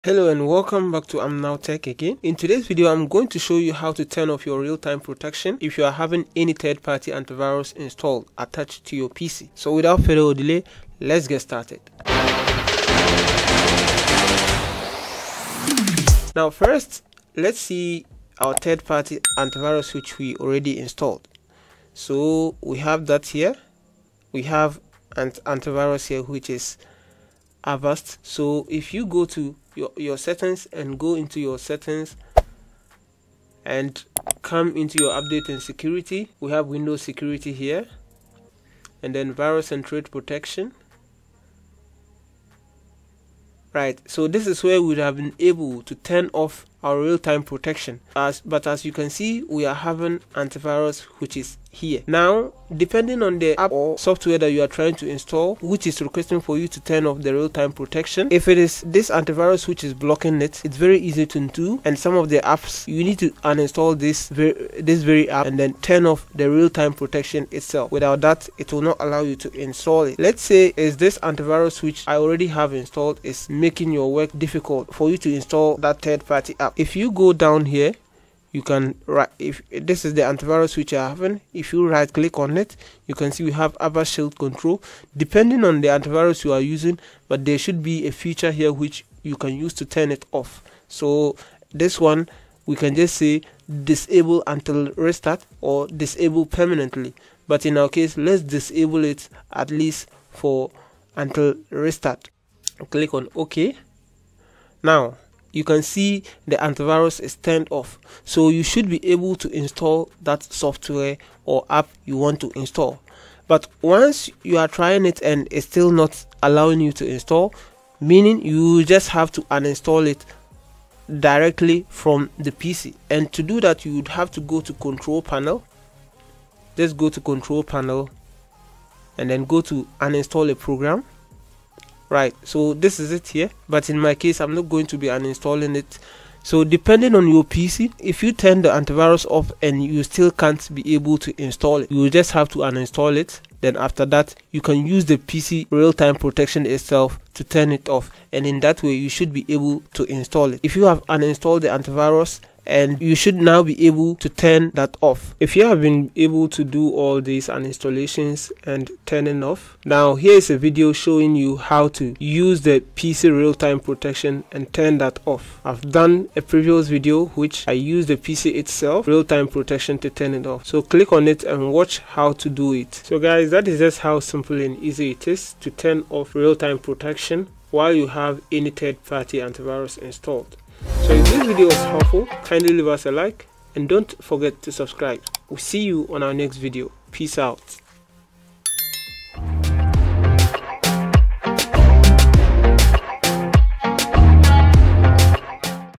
Turn off protection and Install Any Software/App
How to turn off real time protection while third party anti-viruses installed